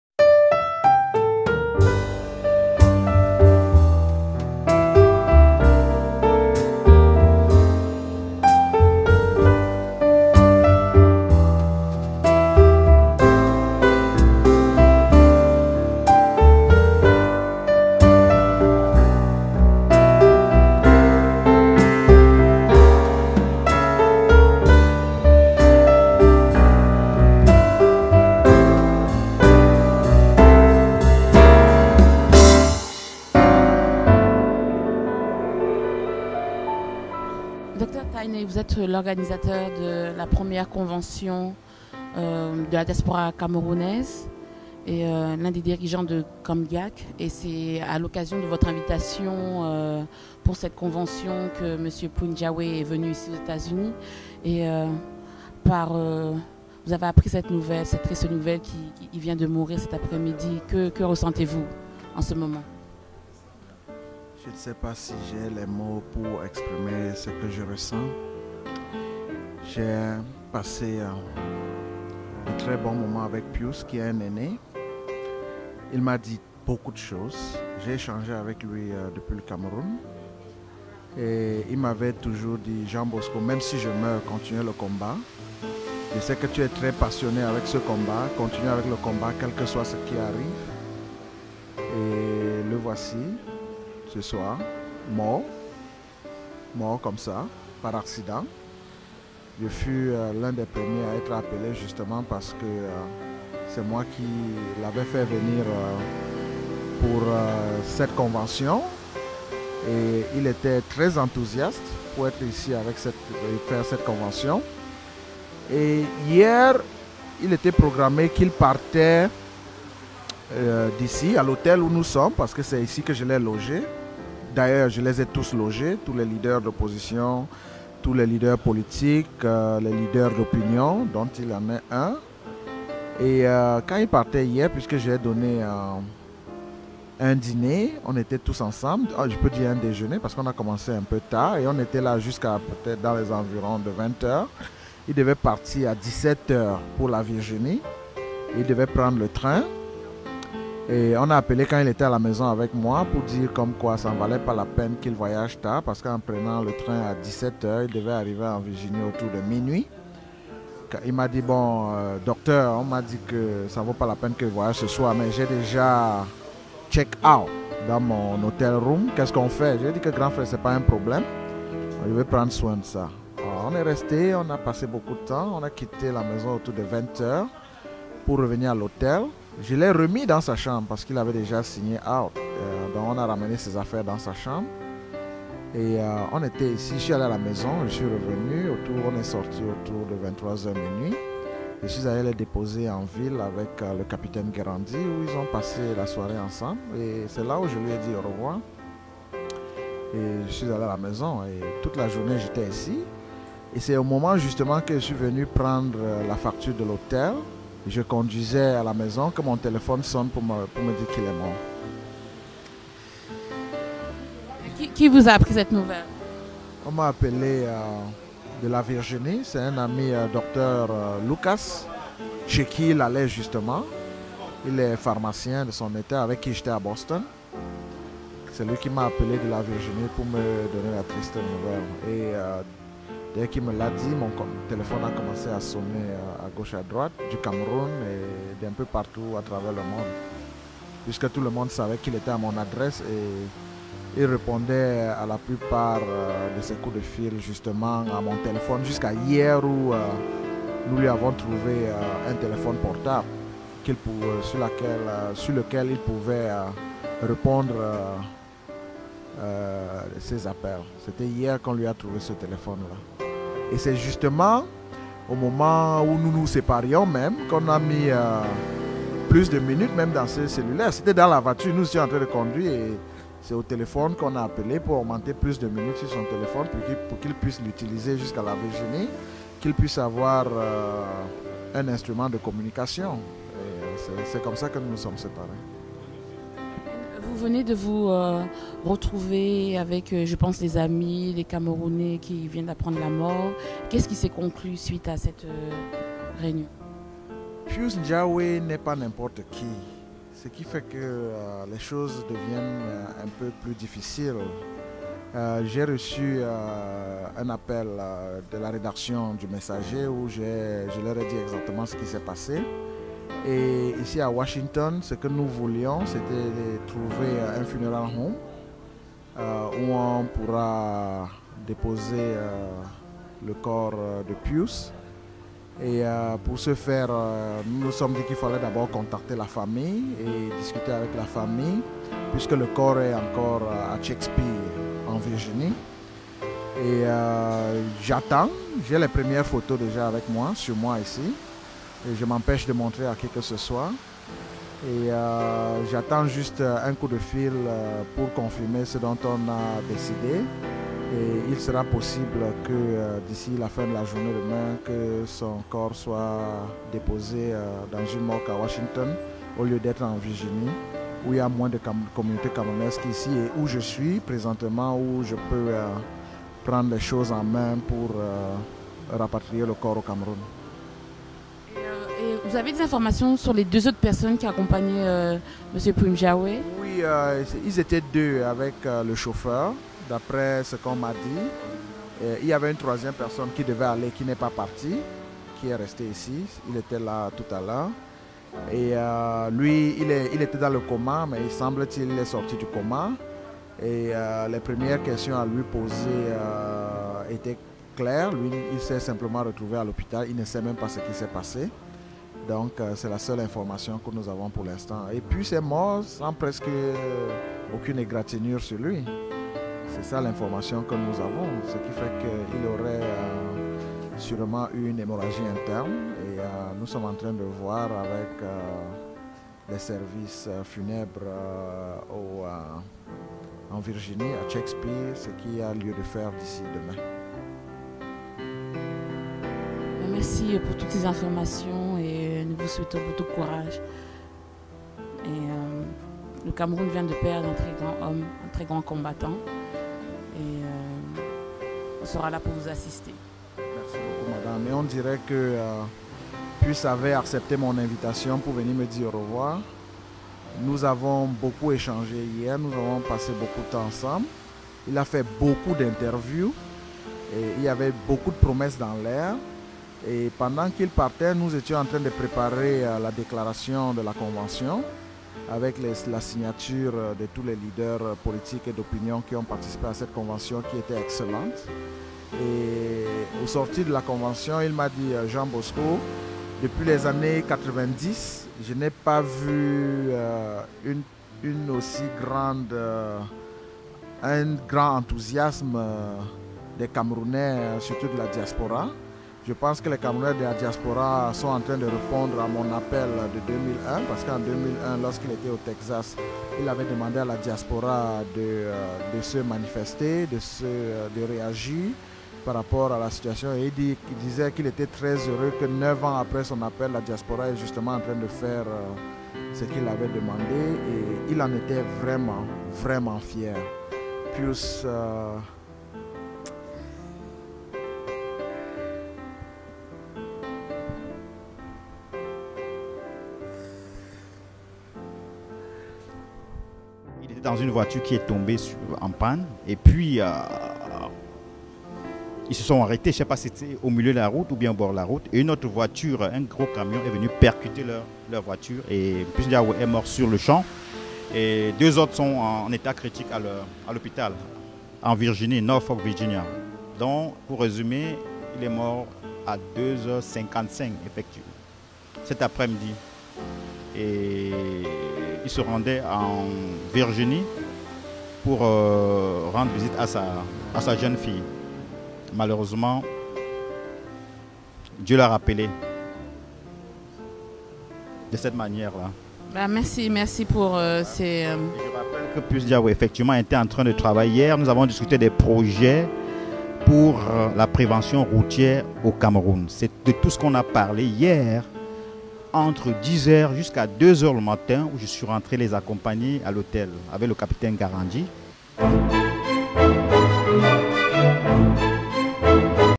les conditions de la mort de Pius
Exclusivite!!! Pius NJAWE decede ce matin en virginie des suites d'un accident de circulation. Realise par Charly Keutch